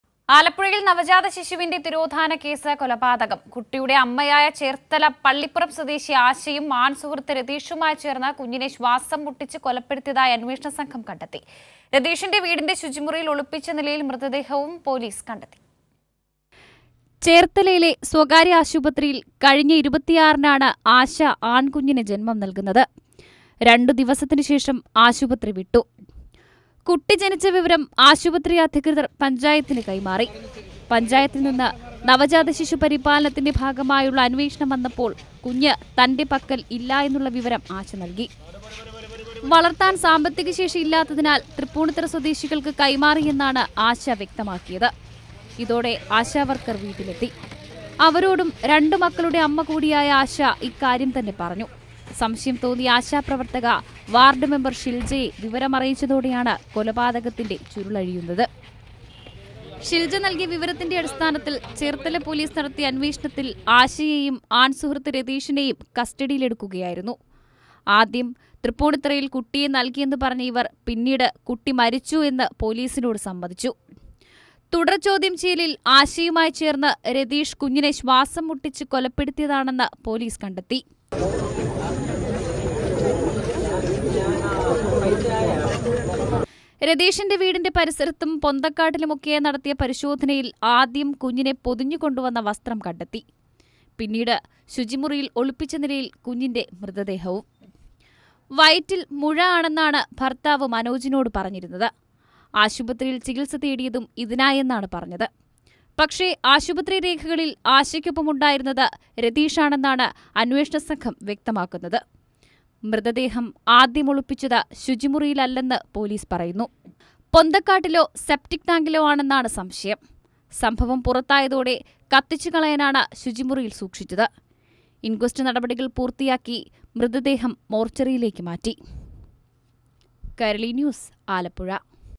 ആലപ്പുഴയിൽ നവജാതശിശുവിന്റെ തിരോധാന കേസ് കൊലപാതകം
ആലപ്പുഴയിൽ നവജാതശിശുവിന്റെ തിരോധാന കേസ് കൊലപാതകം; മൃതദേഹം ശുചിമുറിക്കുള്ളിൽ ഒളിപ്പിച്ച നിലയിൽ
Body of missing newborn baby found in Alappuzha